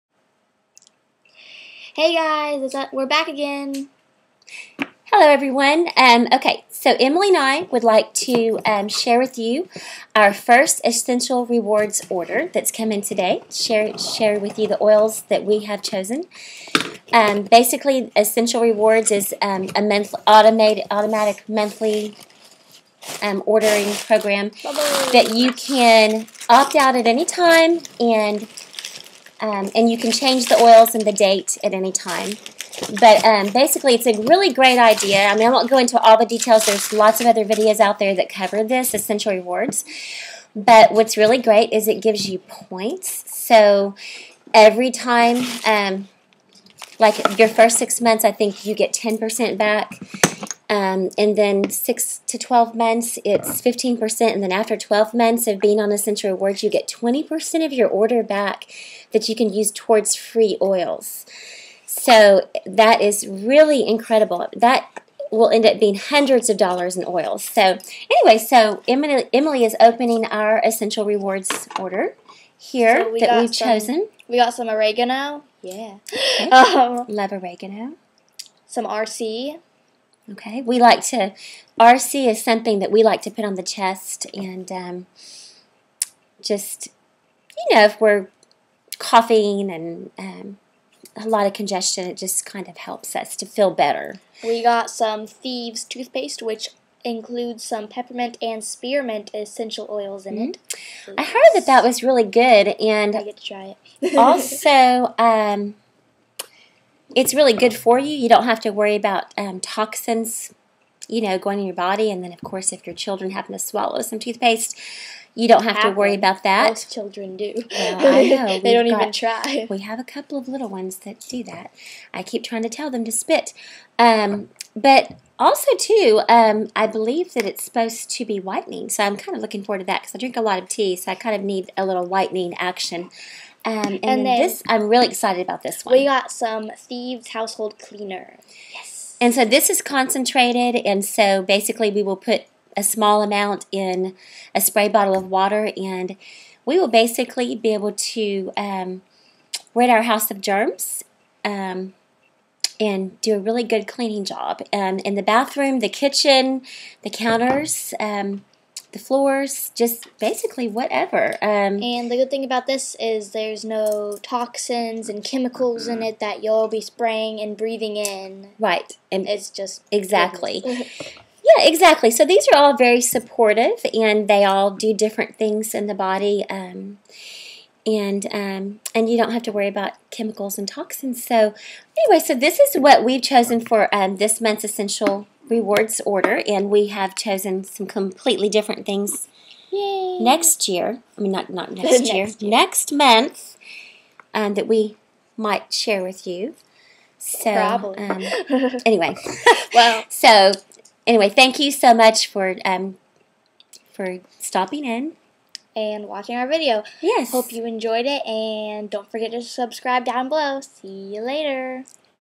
Opening Our First Young Living Essential Rewards Order
Welcome to the Trudy & Emily Young Living Essential Oils mother/daughter team. In this video we are opening our first Essential Rewards Order.